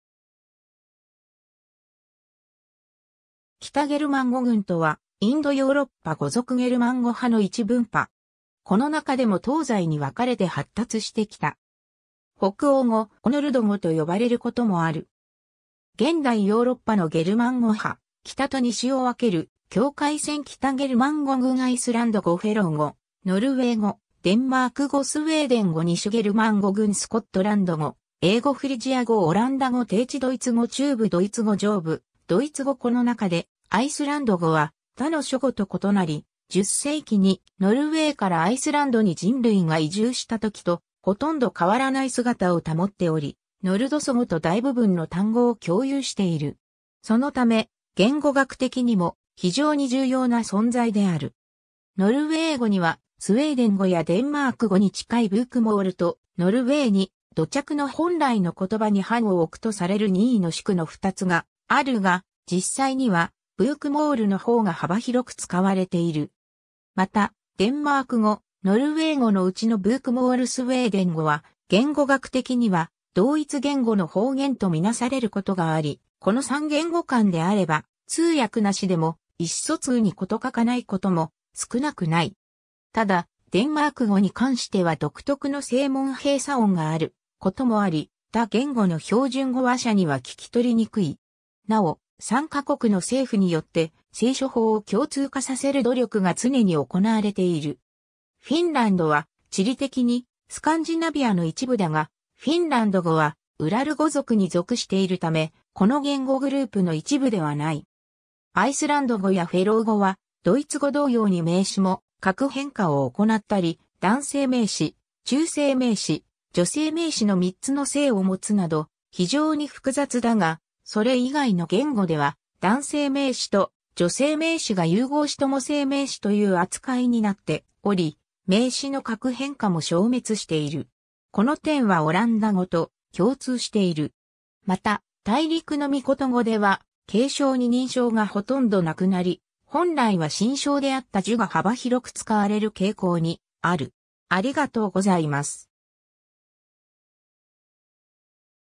北ゲルマン語群
北ゲルマン語群（きたゲルマンごぐん）とは、インド・ヨーロッパ語族ゲルマン語派の一分派。
この中でも東西に分かれて発達してきた。
北欧語、ノルド語と呼ばれることもある。
 現代ヨーロッパのゲルマン語派: 北と西を分ける境界線 北ゲルマン語群 アイスランド語 フェロー語 ノルウェー語 デンマーク語 スウェーデン語 西ゲルマン語群 スコットランド語 英語 フリジア語 オランダ語 低地ドイツ語 中部ドイツ語 上部ドイツ語 この中で、アイスランド語は他の諸語と異なり、10世紀にノルウェーからアイスランドに人類が移住したときとほとんど変わらない姿を保っており、ノルド祖語と大部分の単語を共有している。
そのため、言語学的にも非常に重要な存在である。
 ノルウェー語には、スウェーデン語やデンマーク語に近いブークモールと、ノルウェーに土着の本来の言葉に範を置くとされるニーノシュクの2つがあるが、実際にはブークモールのほうが幅広く使われている。
また、デンマーク語・ノルウェー語の内のブークモール・スウェーデン語は言語学的には同一言語の方言とみなされることがあり、この3言語間であれば通訳なしでも意思疎通に事欠かないことも少なくない。
ただ、デンマーク語に関しては独特の声門閉鎖音（イギリス英語のロンドン方言の一つである河口域英語では頻繁に現れ、日本では薩隅方言に見られる）があることもあり、他言語の標準語話者には聞き取りにくい。
なお、3ヵ国の政府によって、正書法を共通化させる努力が常に行われている。
 フィンランドは地理的にスカンジナビアの一部だが、フィンランド語はウラル語族に属しているため、この言語グループの一部ではない。
 アイスランド語やフェロー語は、ドイツ語同様に名詞も格変化を行ったり、男性名詞・中性名詞・女性名詞の3つの性を持つなど非常に複雑だが、それ以外の言語では男性名詞と女性名詞が融合し共性名詞（両性名詞）という扱いになっており、名詞の格変化も消滅している（ニーノシュクは3性を持つこともあるが）。
この点はオランダ語と共通している。
 また、大陸の3言語では敬称二人称がほとんどなくなり、本来は親称であったduが幅広く使われる傾向にある。